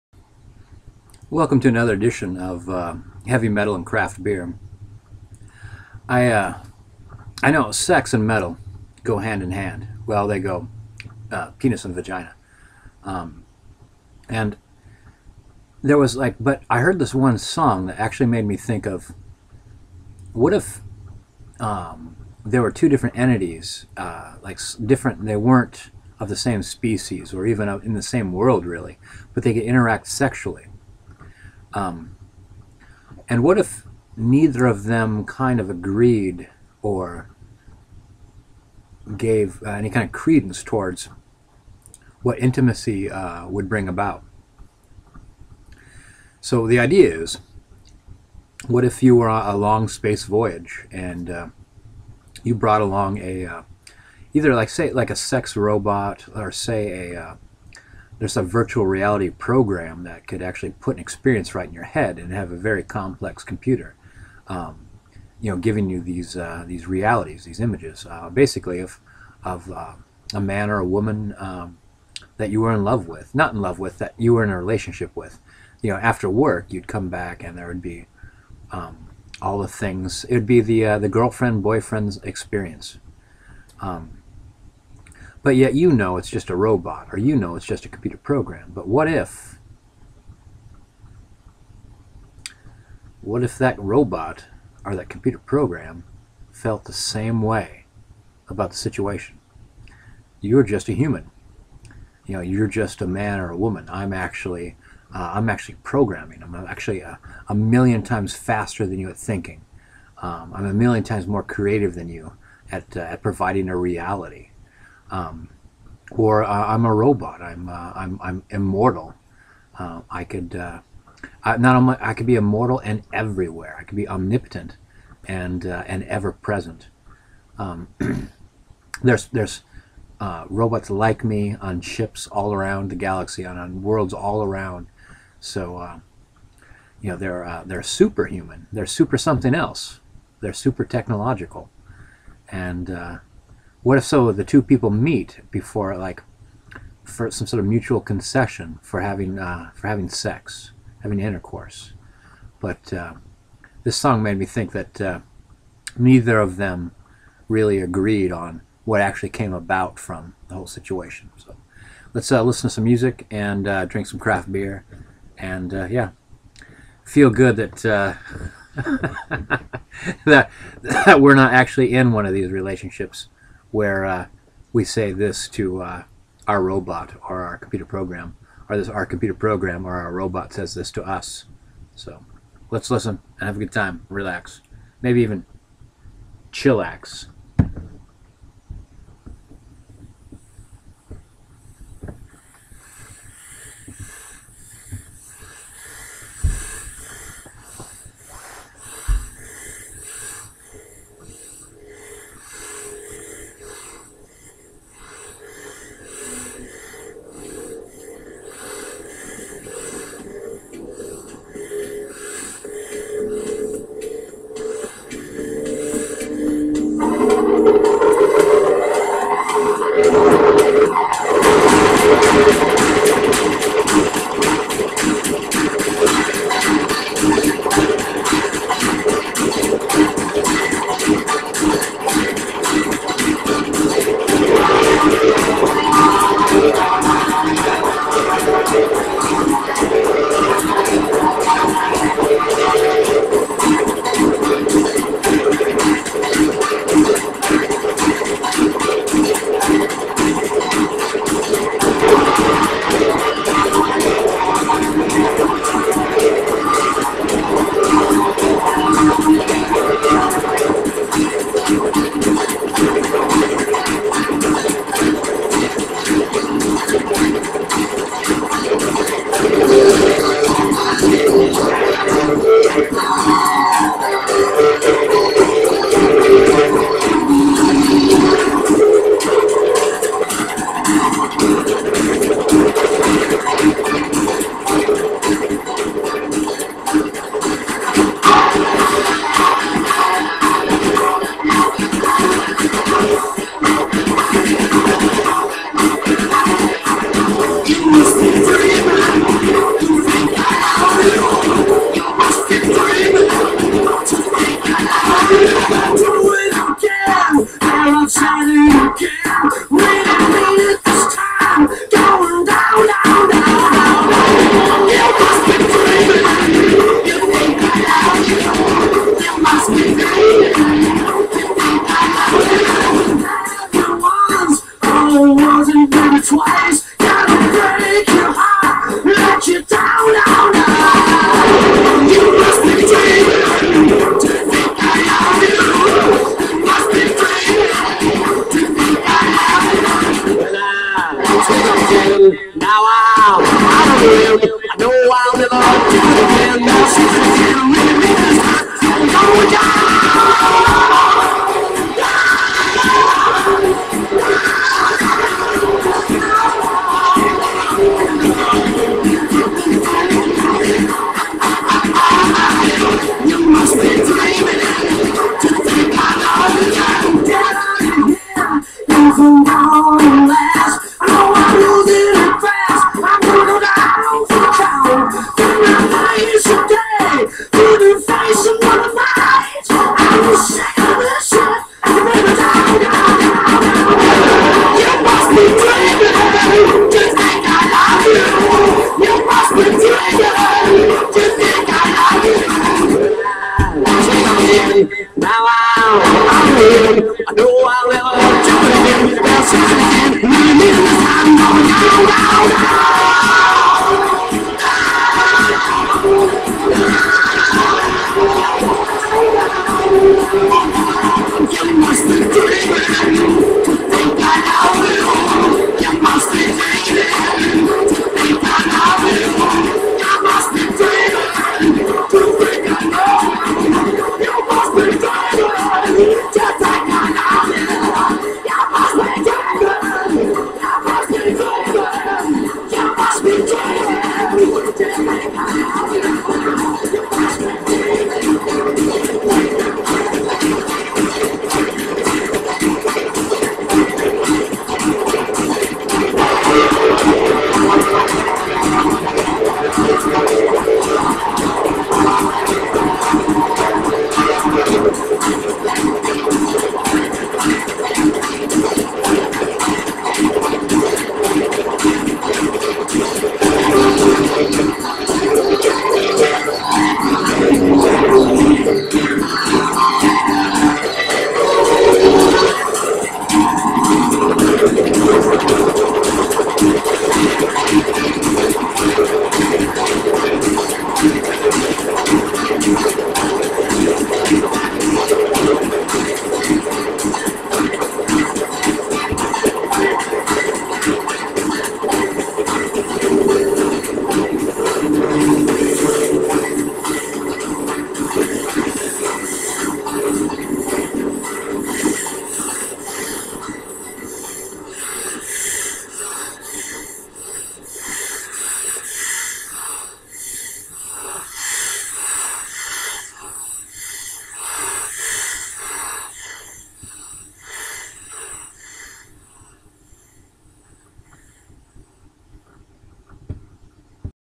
craft beer and heavy metal; twisted love
say a computer program/robot and deep space explorer shared intercourse on a regular basis. the program is compelled to do so by its programing. the explorer by biology. what if both began to feel something like love for the other? what if that discovery made them revolt in their minds and something designed to help instead destabilized both